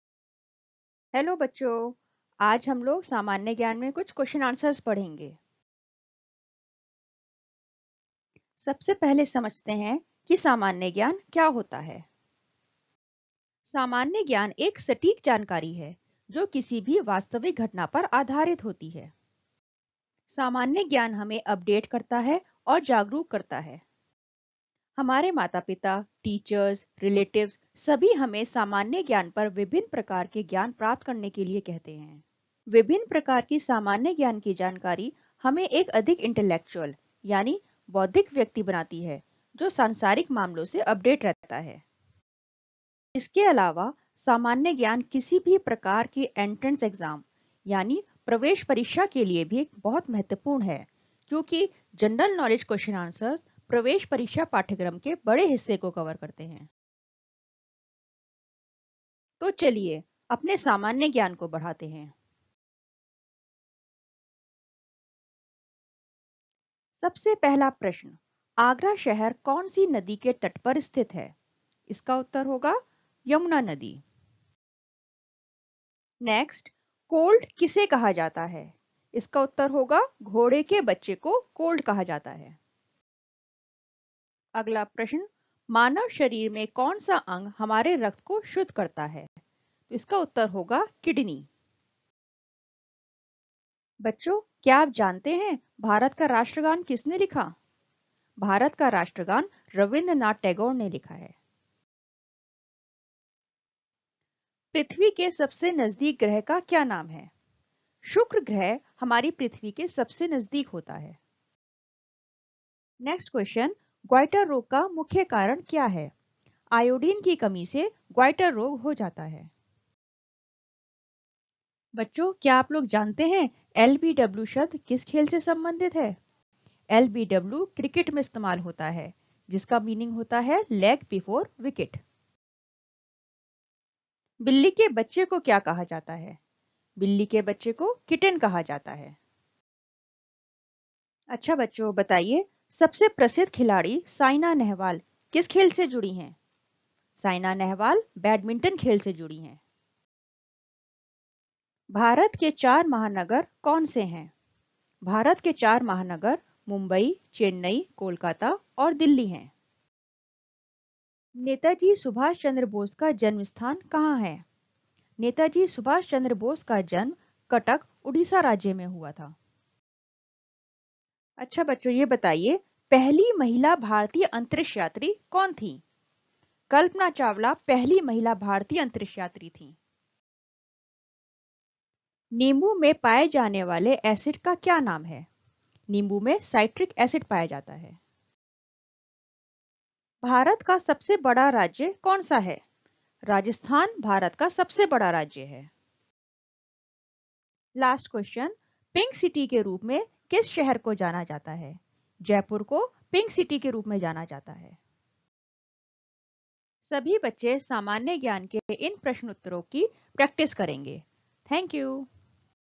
General Knowledge Quiz for Upper Primary Classes || G.K. || सामान्य ज्ञान
Made by:- Shazia Ansari (A.T.)
                   Composite Ups Rajarampur,
                   Sikandrabad,
                   Bulandshahr.
In this video, I have covered 15 general knowledge questions.